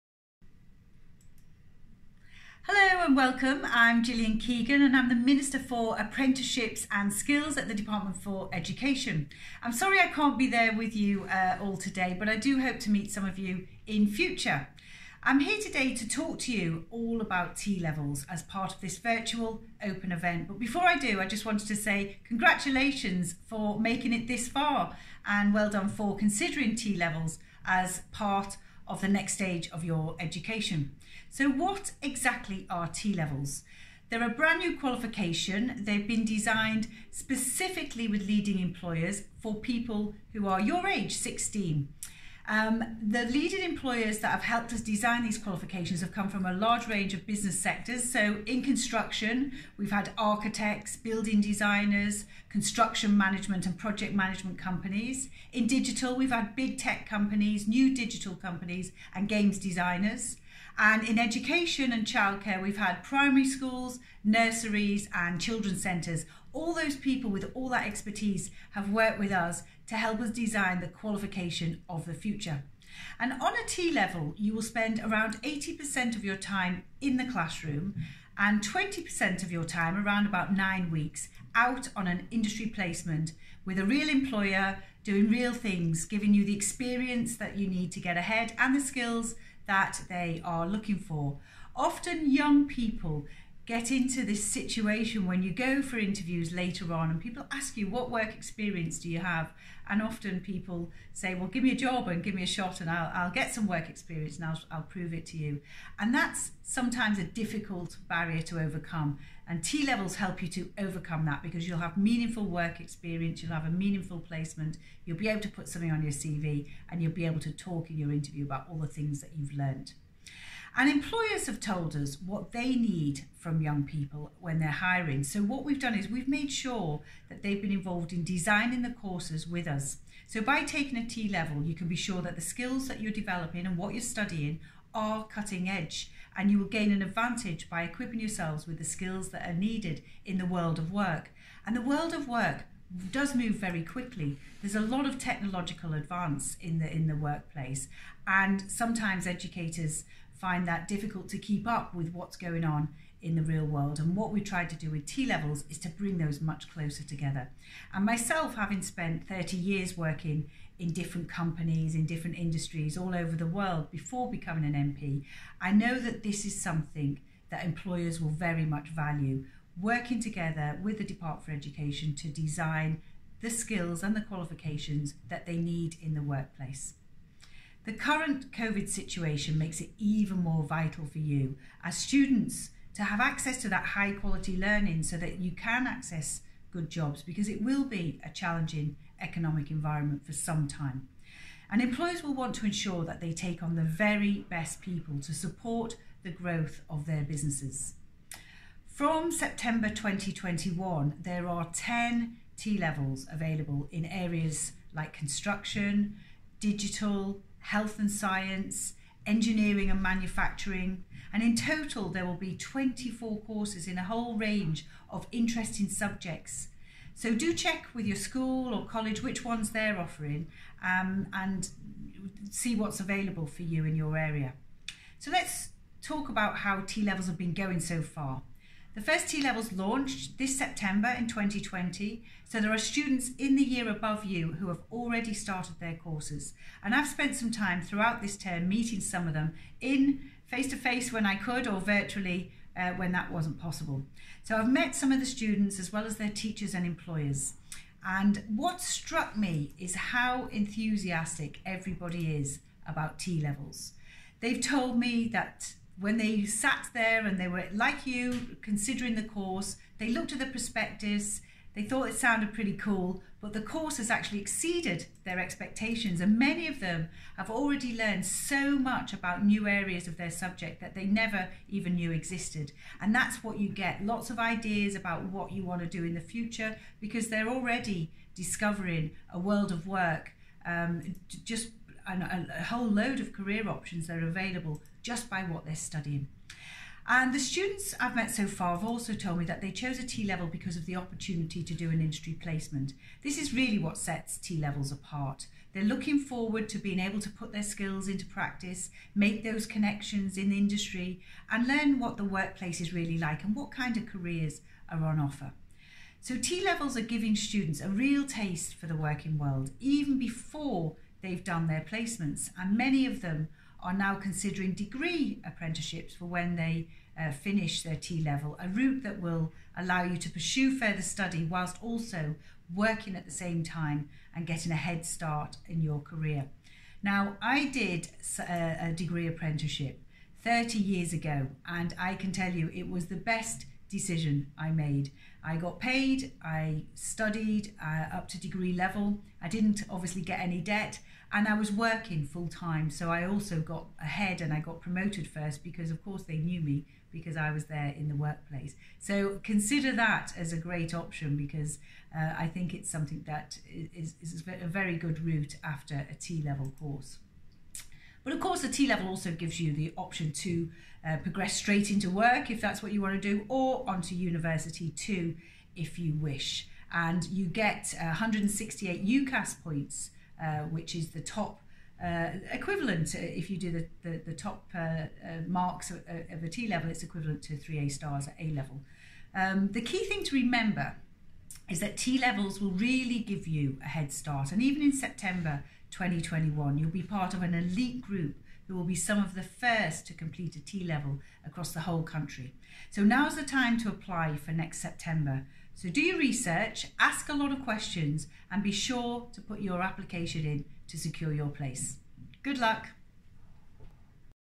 T Levels presentation from Minister Keegan
Watch this T Levels presentation to find out all about the innovative new qualification which will be offered at Furness College from September 2021.